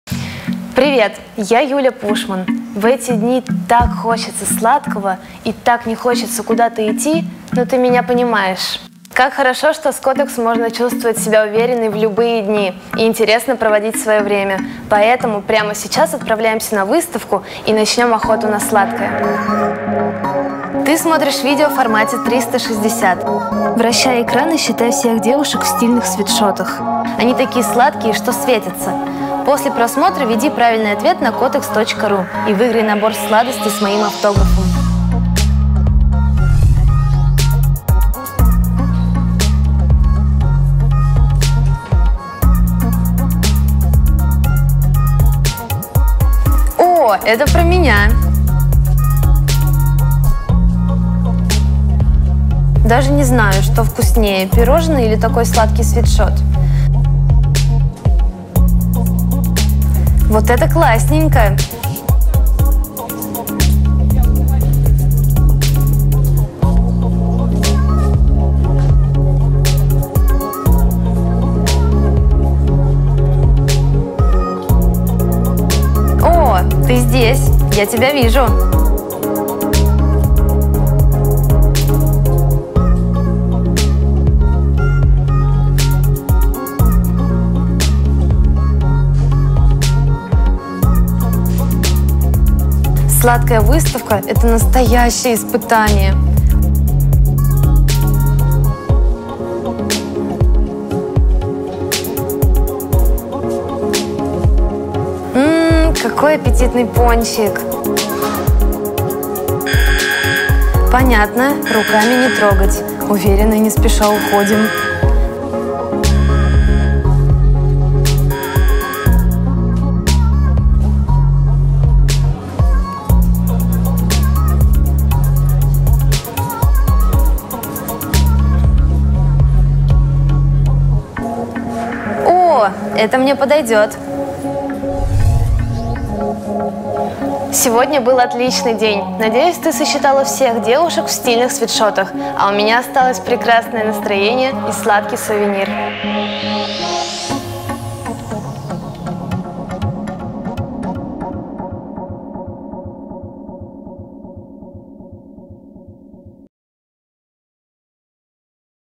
360° Kotex-квест с Юлией Пушман
Разгадай уникальный Kotex-квест в формате 360°!
Сосчитай всех девушек в стильных свитшотах и выиграй наборы сладостей с автографом Юлии Пушман! Ждем твои ответы на нашем сайте